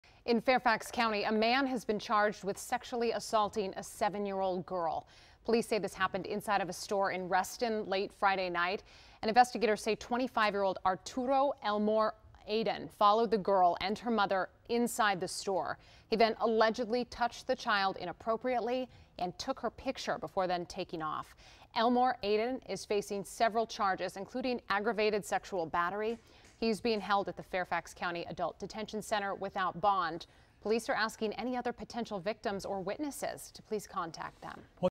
Man arrested for sexually assaulting young girl in Fairfax County
In Fairfax County, a man has been charged with sexually assaulting a 7 year old girl.